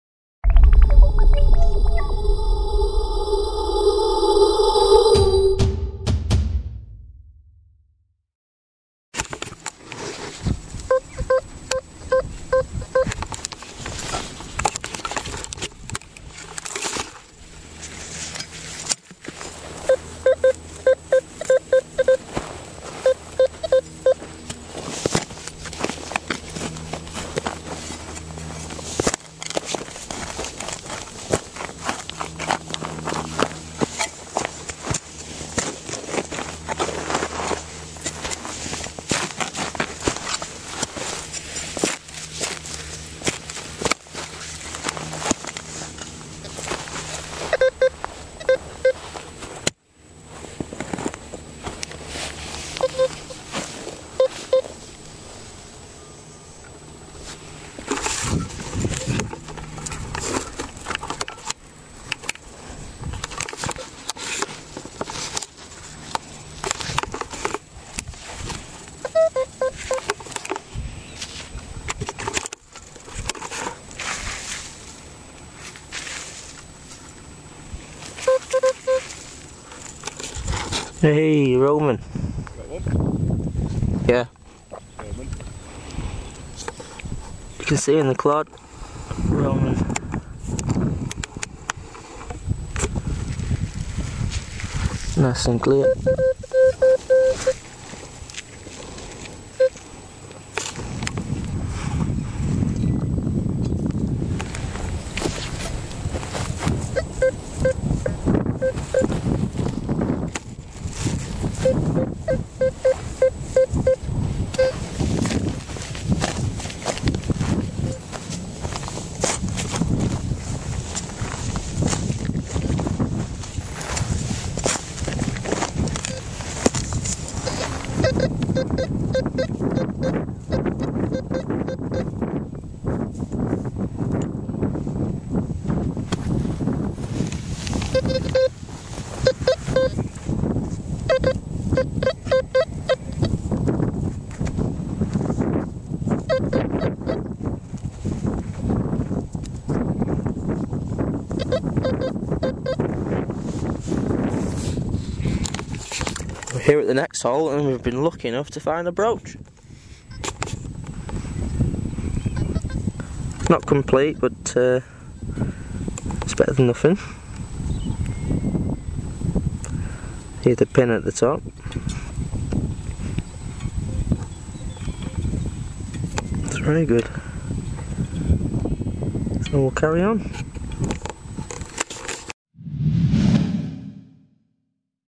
Metal Detecting Uk - Finding a Roman coin and Brooch
Metal Detecting Uk - Finding a Roman coin and Brooch with an XP Goldmaxx Power, using the elipitical coil.